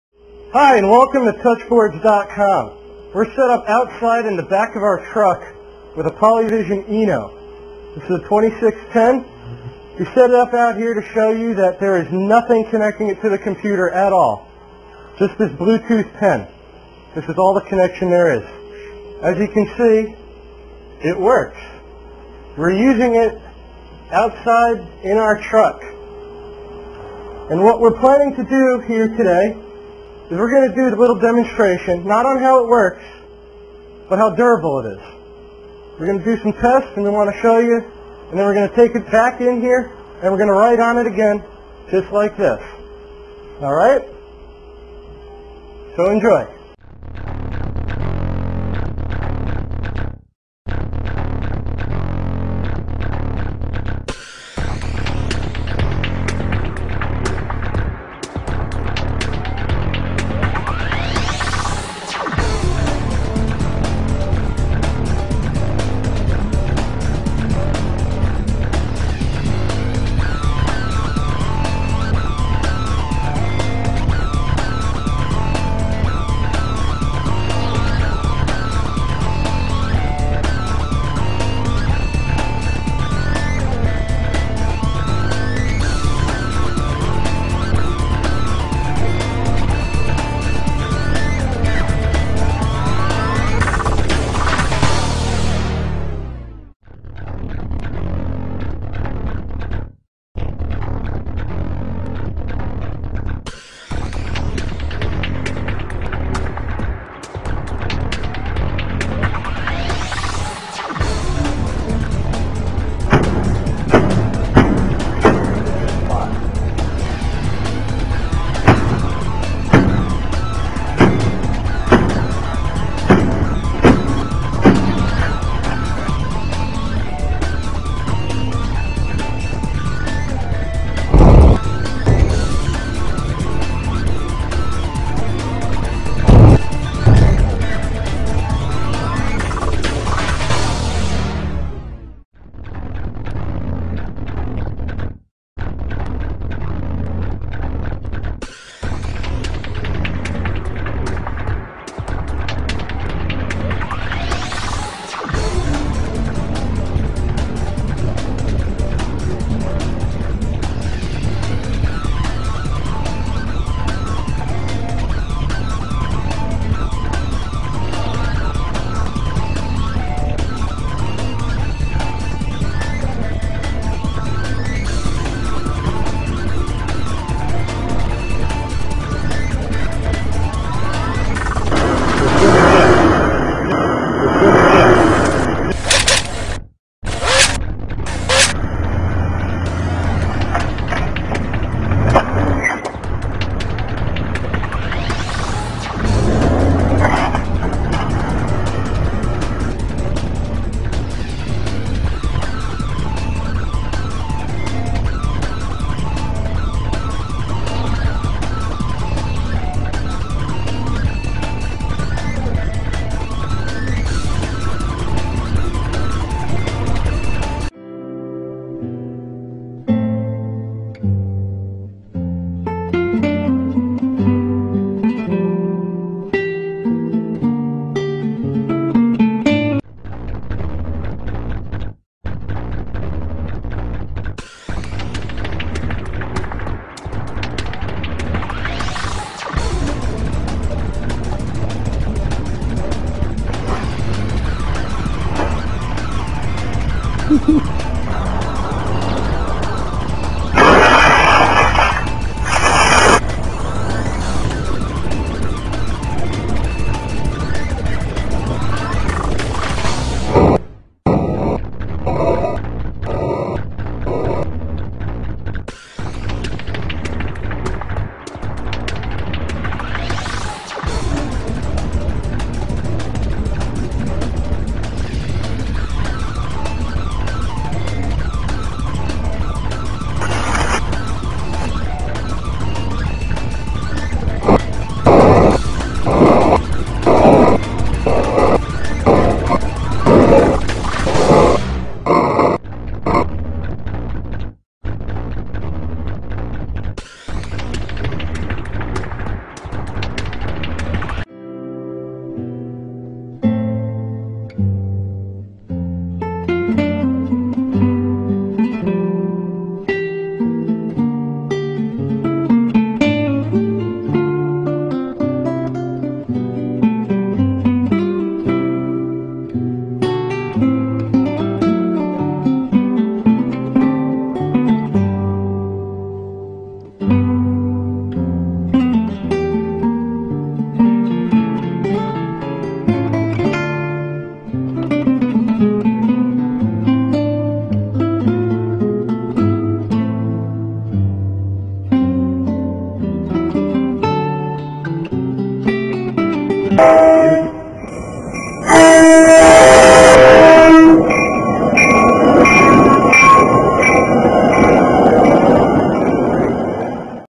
Интерактивная доска ТМ 2х3 "Eno Classic"
Доска интерактивная ТМ 2х3 " Eno Classic ". (для приобретения даного товара необходимо сделать предварительный заказ по тел. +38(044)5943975 (Киев, Украина). Подробности на сайте интернет-магазина Папирус
Размер доски 160x120 см с магнитно-маркерной поверхностью, обладает лучшими свойствами керамической доски и интерактивной. Поверхность имеет большую устойчивость ко всякого рода повреждений таких как: рисование, стирание, краска в спрее, огонь, грязь. Доска eno отличается от других интерактивных досок тем, что она не имеет соединительных проводов, дает возможность свободного передвижения около доски. Доска работает с любым доступным на рынке программным обеспечением. Доска не требует никакого питания, в ней нет никакой электроники. Для работы с интерактивным функционалом применяется специальный инструмент - электронный маркер (стилус) Bluetooth. На поверхность доски нанесен серый слой координат. В перо стилуса встроенная камера и микропроцессор, считывающее слой координат. В свою очередь взаимодействие с компьютером осуществляется через Bluetooth встроенный в стилус. Преимущества доски ЕNO Сlassic: - Сверхпрочная и надежная конструкция; - Для электропитания и передачи данных не используются кабель, (простая установка и безопасная эксплуатация); - Для установки требуется 1 человек и практически сразу можно начинать без какого-либо предварительного обучения - гибкое и удобное управление функционалом позволяет с легкостью без потери времени переходить от простых инструментов к интерактивным, переключаться между различными компьютерными программами и управлять их возможностями, осуществлять навигацию в интернет-пространстве - полный контроль над информацией. Для работы стилуса необходима батарейка. В ПОДАРОК к доске вы получите программное обеспечение WizTich.